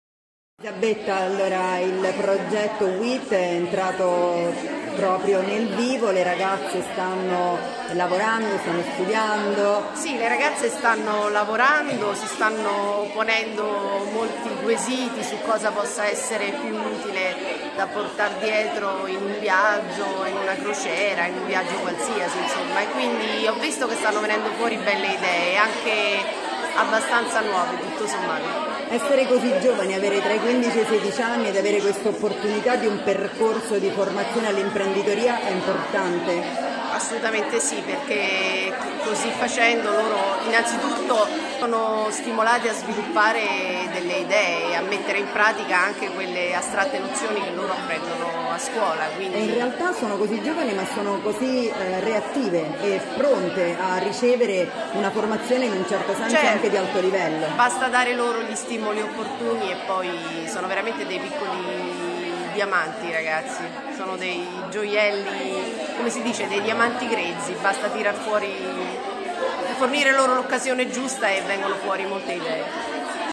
WIT, la docente Durantini: "le ragazze sono piccoli diamanti"
La professoressa Elisabetta Durantini docente coordinatrice del progetto WIT nella scuola LSS Vito Volterra di Ciampino. Dare alle ragazze la possibilità di cimentarsi in attività diverse dalla didattica tradizionale gli permette di esternare tutte le potenzialità che hanno.

Il progetto Women in Technology (acronimo WIT) è dedicato a 150 studentesse di Campania, Calabria e Sicilia, per prevenire il fenomeno dei Neet e creare nuove opportunità di lavoro. Il liceo Vito Volterra di Ciampino è la scuola pilota, dove vengono sperimentate in anteprima innovative metodologie didattiche.